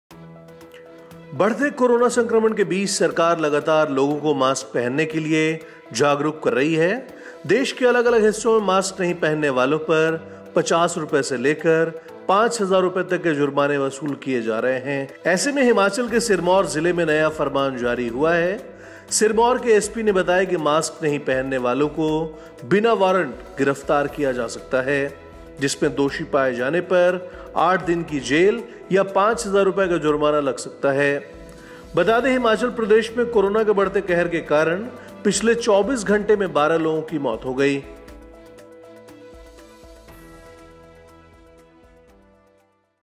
हिमाचल: सिरमौर में mask नहीं पहनने वालों की होगी बिना वारंट गिरफ्तारी
बढ़ते कोरोना संक्रमण के बीच सरकार लगातार लोगों को मास्क पहनने के लिए जागरूक कर रही है. देश के अलग-अलग हिस्सों में मास्क नहीं पहनने वालों पर ₹50 से लेकर ₹5000 तक के जुर्माने वसूल किए जा रहे हैं. ऐसे में हिमाचल के सिरमौर जिले में नया फरमान जारी हुआ है. सिरमौर के एसपी ने बताया कि मास्क नहीं पहनने वालों को बिना वारंट गिरफ्तार किया जा सकता है. जिसमें दोषी पाए जाने पर 8 दिन की जेल या ₹5000 का जुर्माना लग सकता है. बता दें कि हिमाचल प्रदेश में कोरोना के बढ़ते कहर के कारण पिछले 24 घंटे में 12 लोगों की मौत हो गई है.